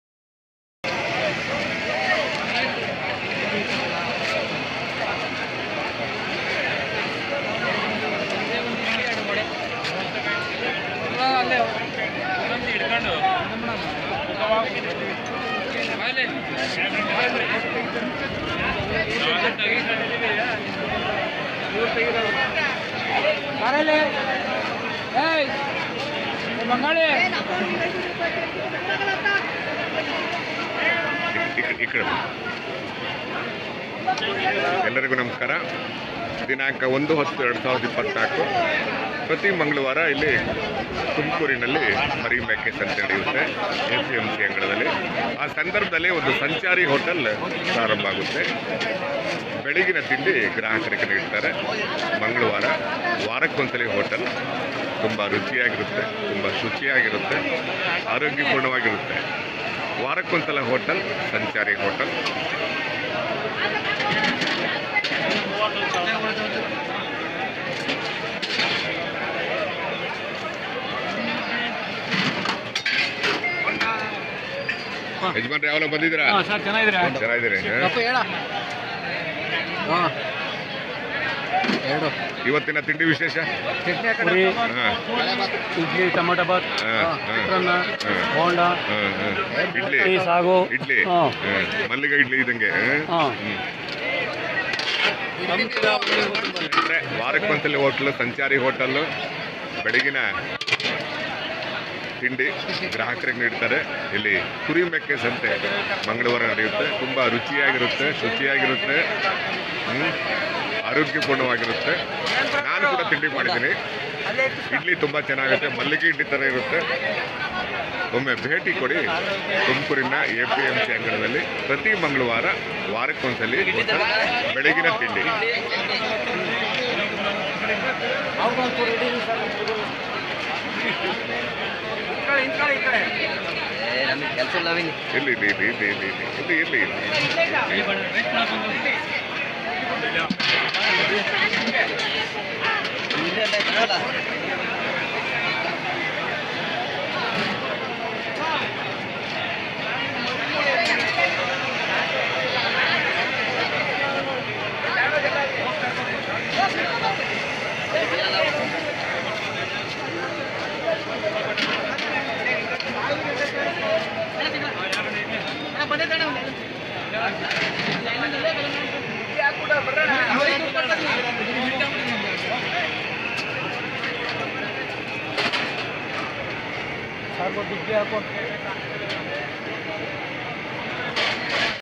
ಸಂಚಾರಿ ಹೋಟೆಲ್ ತುಮಕೂರು ಮರಿ ಸಂತೆ ಯಲ್ಲಿ
Sanchari hotel exist in sheep and goats market at tumakur apmc yard